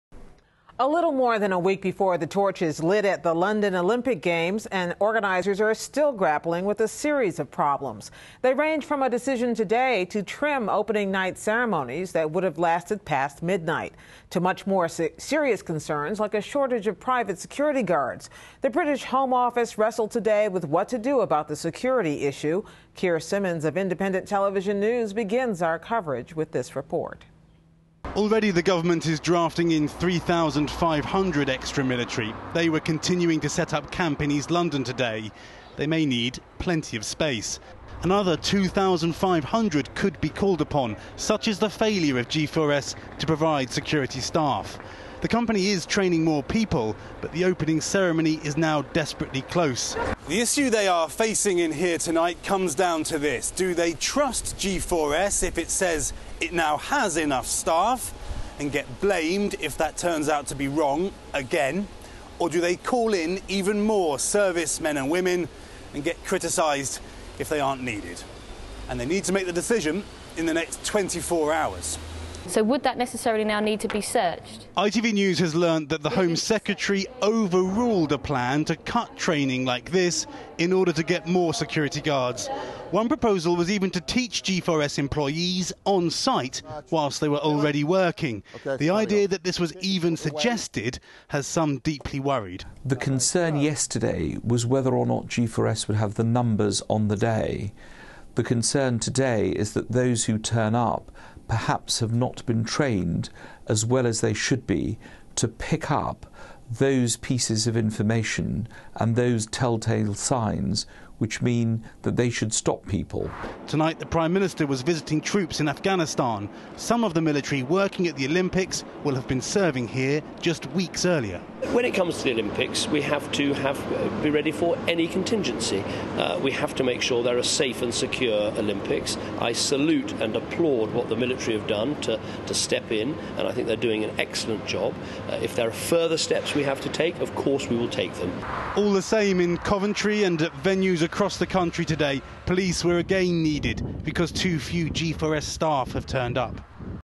Olympics Organizers Grapple with Security Concerns
Organizers of the London Olympic Games face security concerns and unfinished opening ceremonies with only nine more days to go. Keir Simmons of Independent Television News has a report.